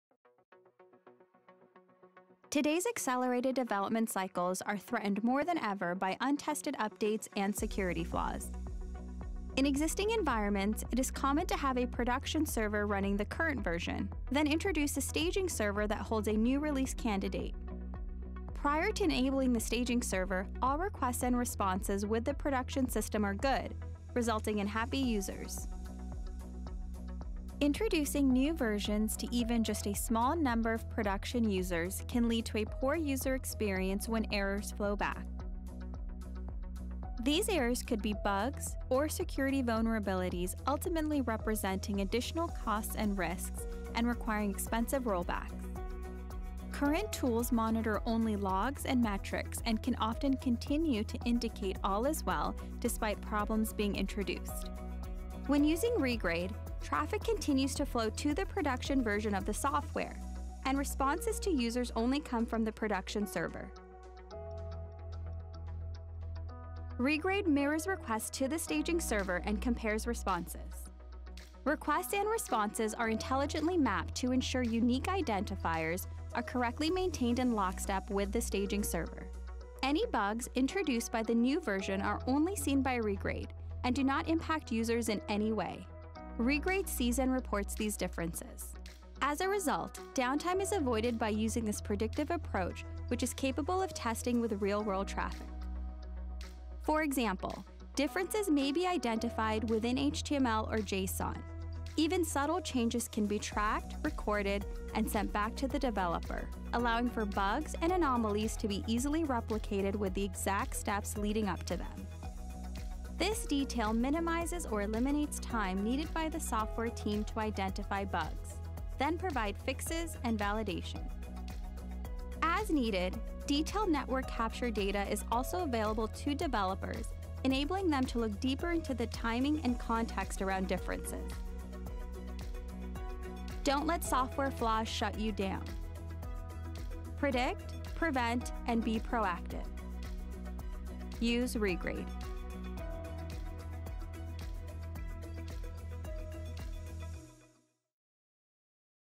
ReGrade How it works in 2 minutes.
Test less.  Cover more.  Know the future before it happens.

Tom Cruise in the Minority Report sci-fi movie had psychics called “PreCogs” that apprehended criminals based on foreknowledge, before the actual committing of the murder. If you are a fan you should hear this out. That’s what do for software.

digitalprecognition A better way to test.

• Eliminate surprises in production, poor user experiences, and costly rollbacks by comparing application behavior before going live
• Detect defects at all layers (not just front end) to automatically pinpoint issues to address
• Save developer time by automatically finding defects and replaying traffic to validate fixes
• Without writing more tests make full regression testing easier and broader
• See differences between UAT/Non-Prod and Prod quickly